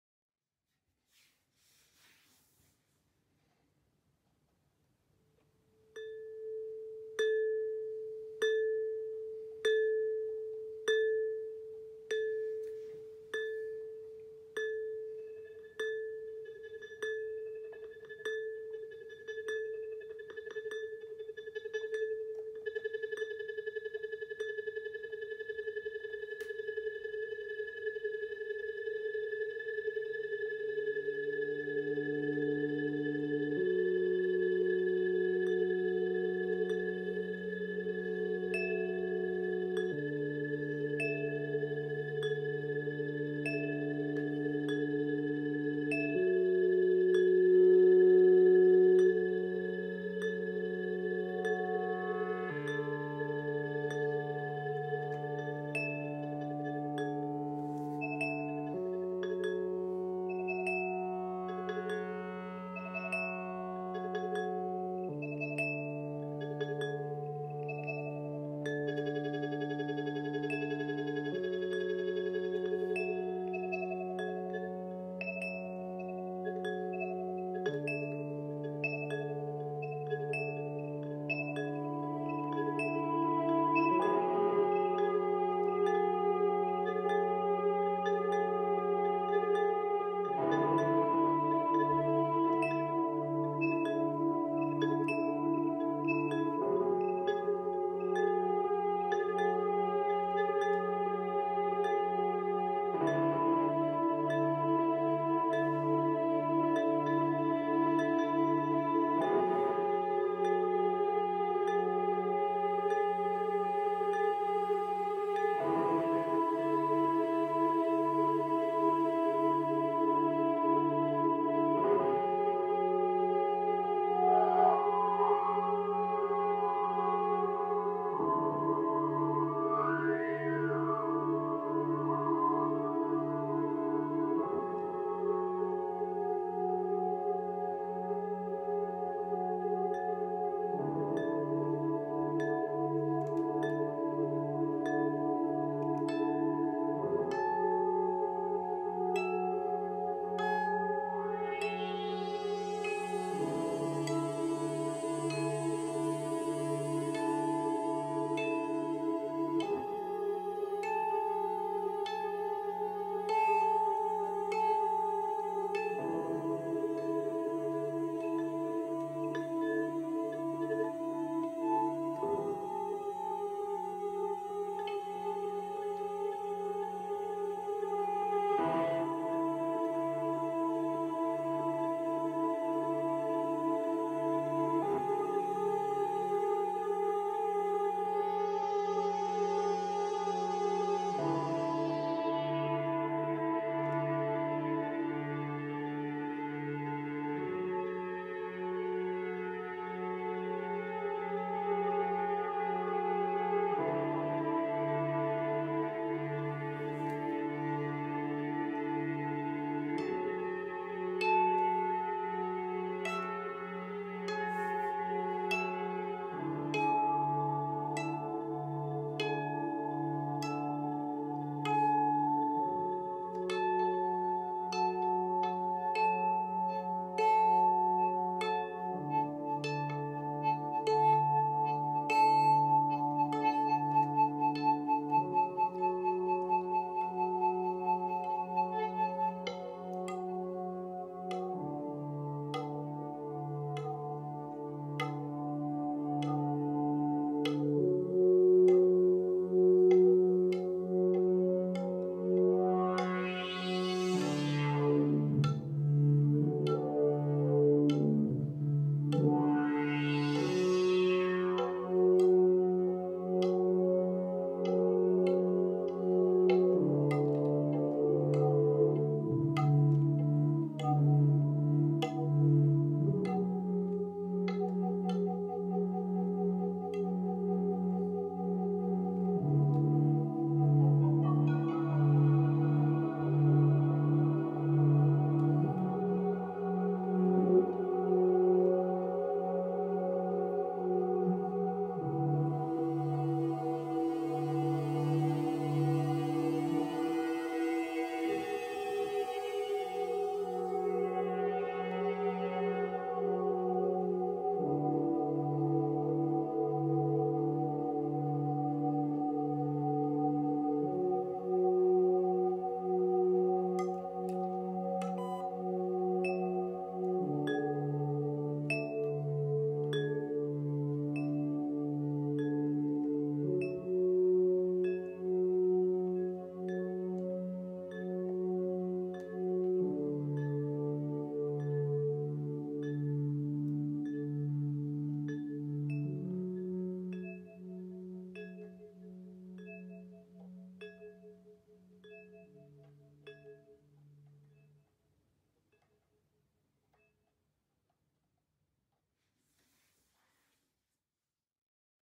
Ambient Electronic Music on a Modular Synthesizer
A short 4 notes sequence of Rings into Clouds accompanied by a couple of moving drones. This is my first Ambient piece performed entirely on a modular system.